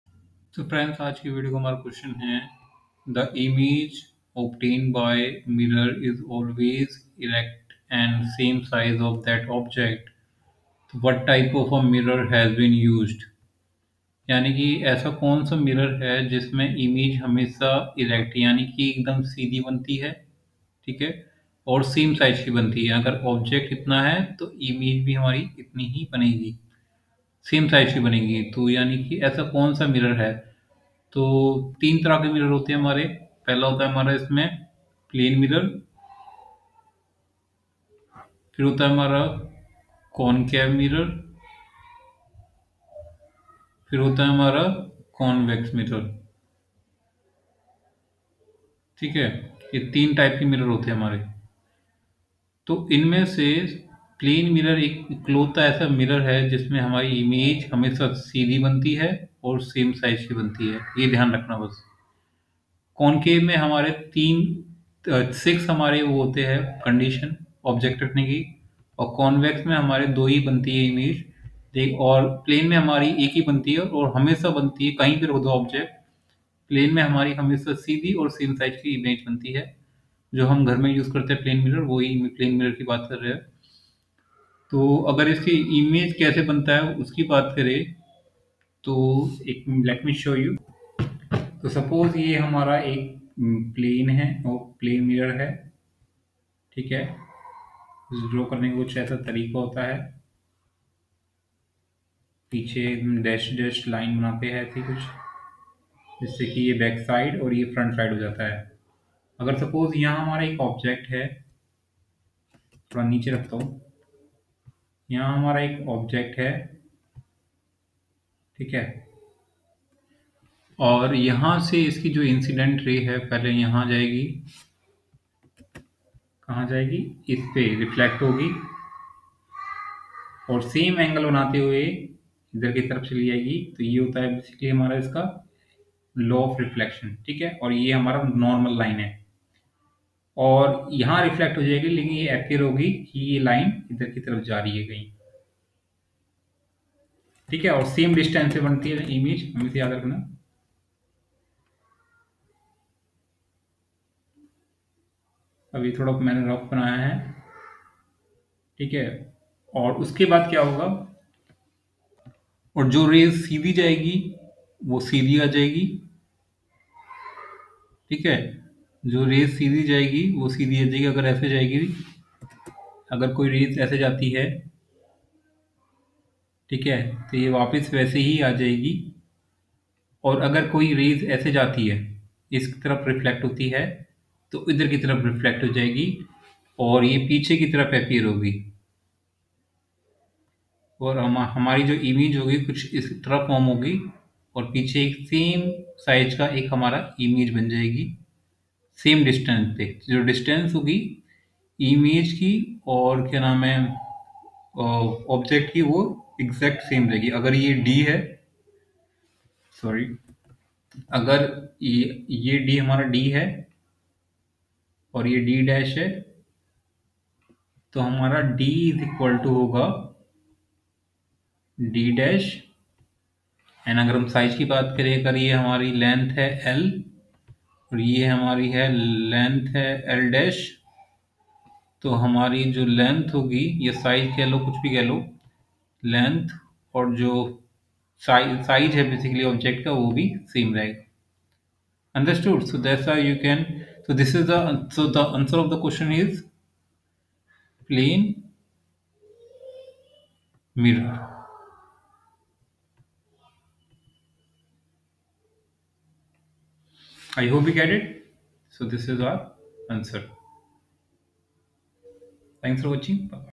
The image obtained by a mirror is always erect and of the same size. Which mirror is used?
the image obtained by a mirror is always erect and of the same size as that of the object. what type of mirror has been used?
Connect with via other platforms👥